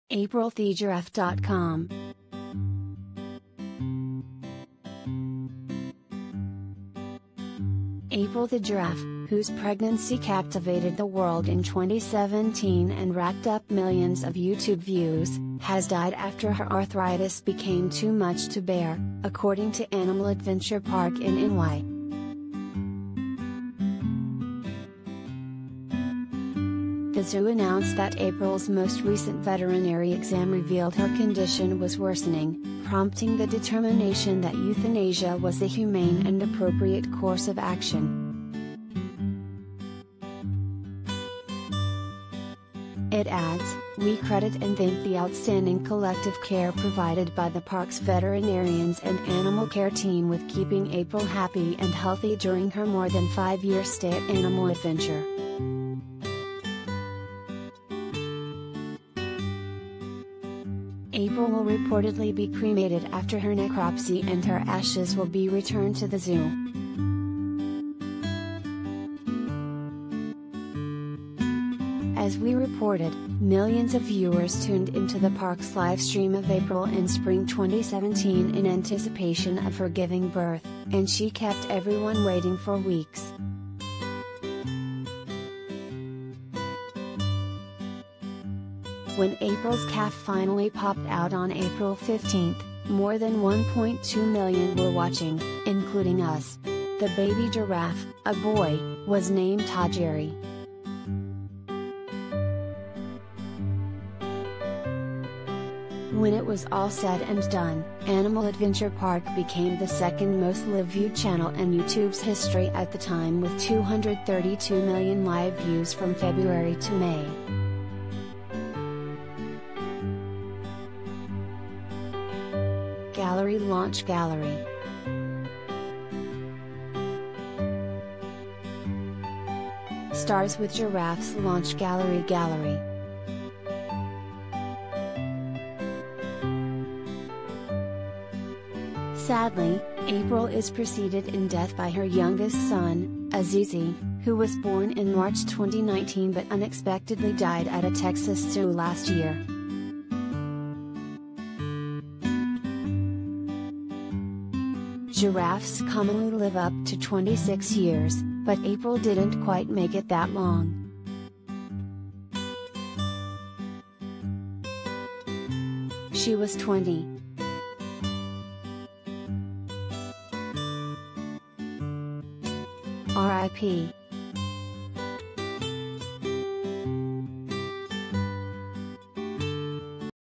April the Giraffe, Viral Sensation of 2017, Dies at 20
April the Giraffe, whose pregnancy captivated the world in 2017 and racked up millions of YouTube views, has died after her arthritis became too much to bear ... according to Animal Adventure Park in NY.
The zoo announced that April's most recent veterinary exam revealed her